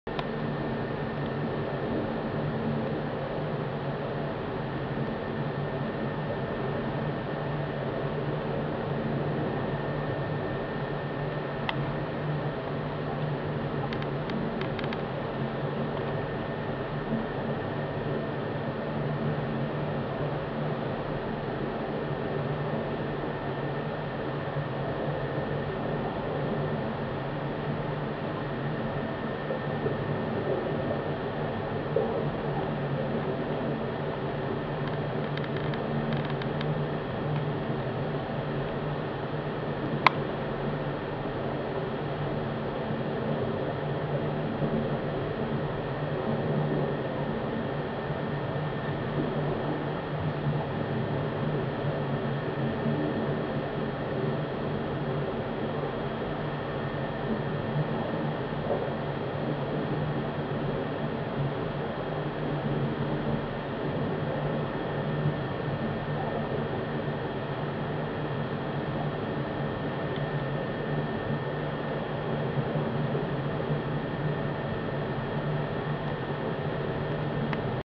Toulouse, France Power Plant Water
an old power plant, now partially unused, houses Electricite de France exhibits and allows visiting the formerly underwater galleries upstream of the turbines